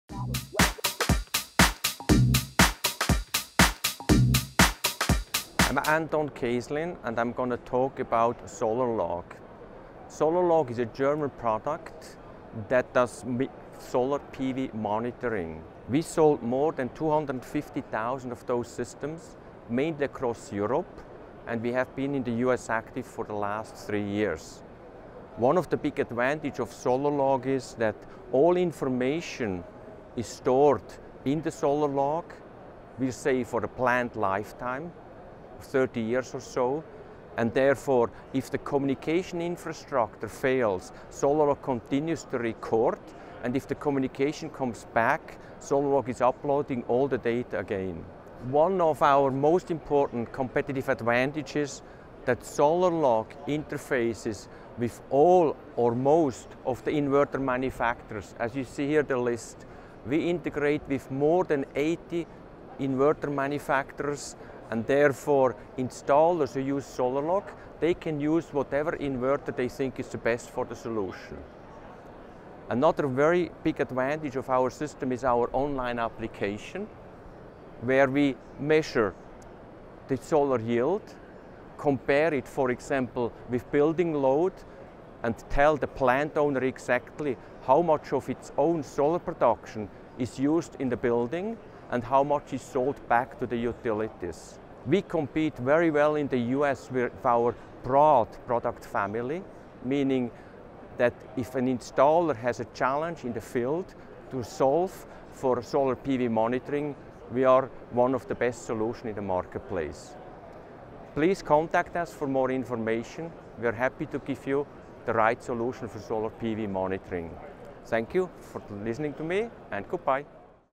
SolarLog - SPI 2013
SolarLog shows Solar Power World their SolarLog technology at Solar Power International 2013.